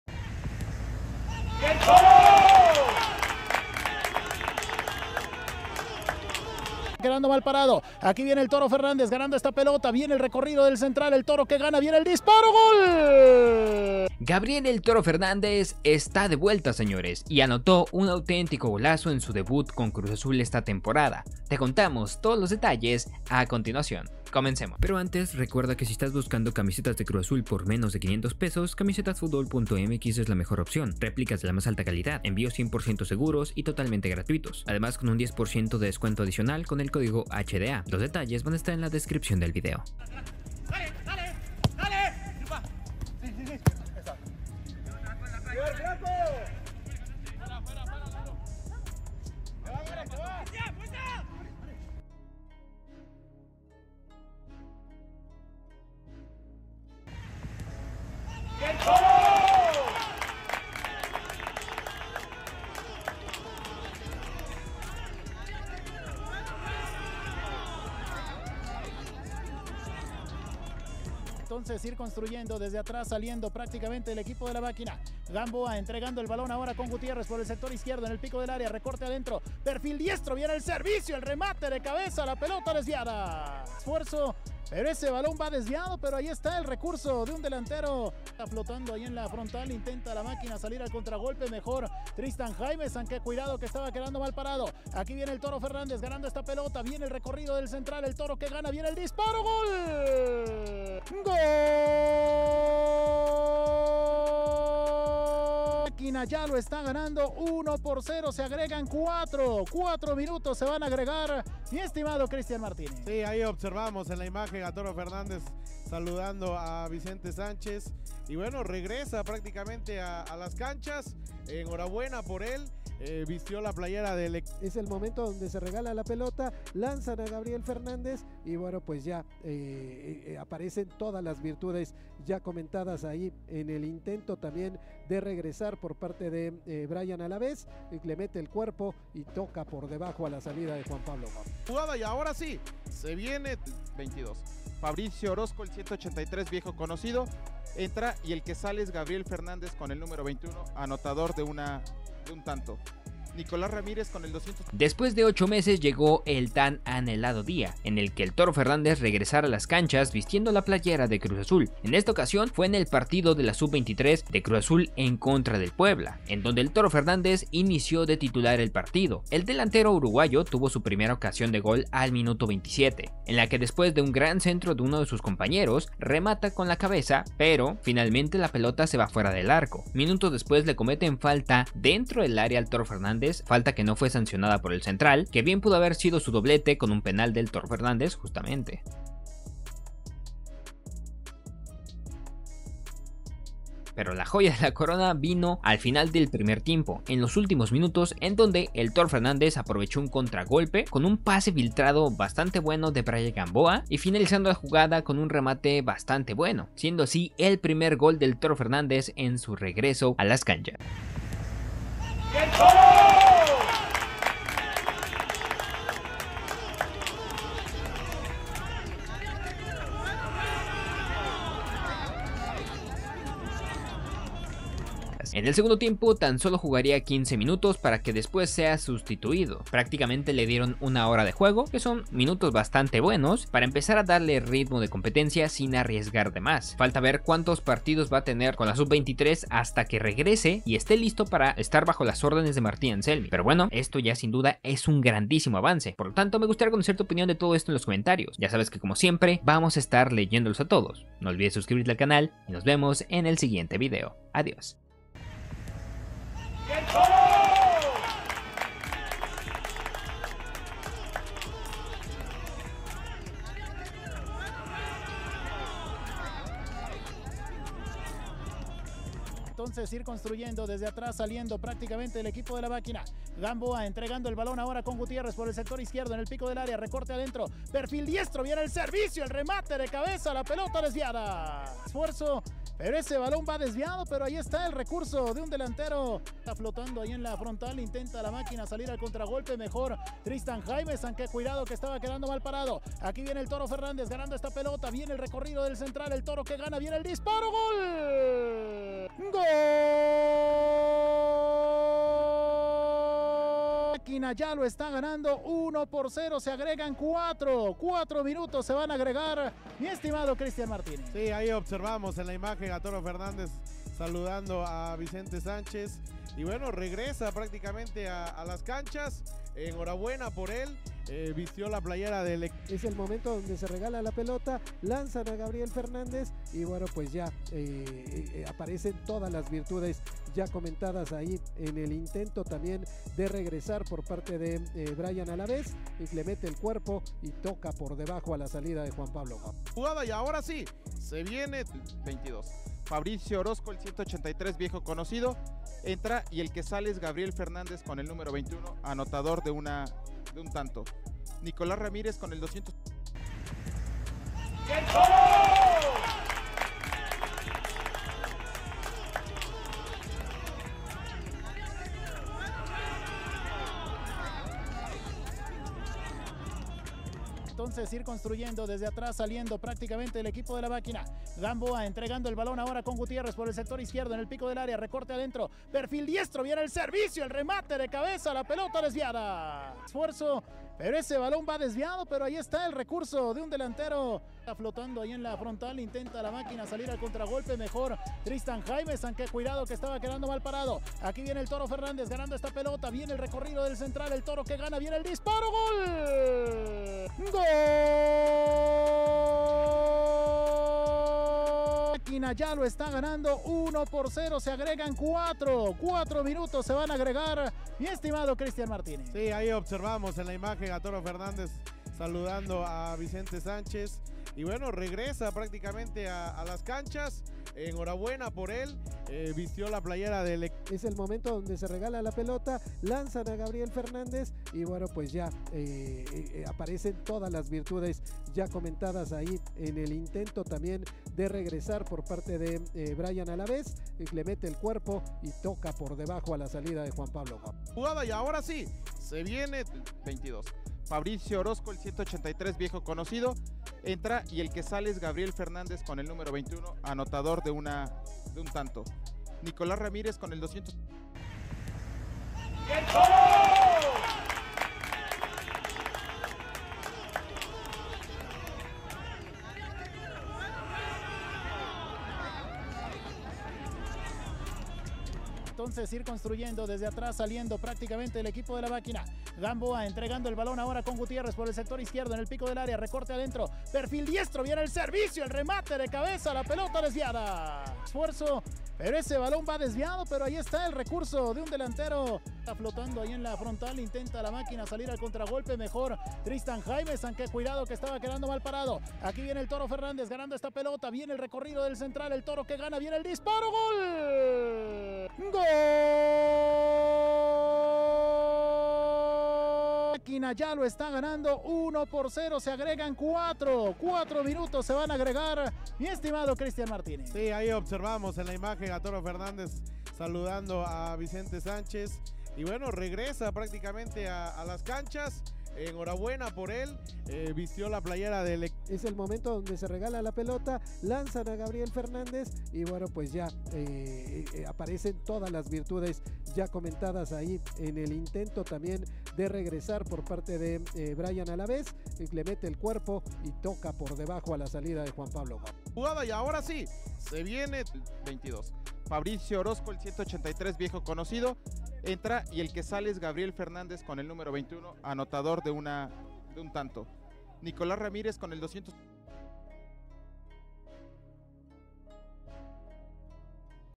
ASÍ FUE EL REGRESO DEL TORO FERNÁNDEZ con CRUZ AZUL ¡ANOTÓ GOL Y ESTÁ LISTO PARA VOLVER!
¡ATENCIÓN AZULES!

Gabriel "El Toro" Fernández está de regreso con Cruz Azul. El delantero uruguayo tuvo sus primeros minutos después de una lesión de 8 meses. El Toro jugó 60 minutos y anotó su primer gol con Cruz Azul esta temporada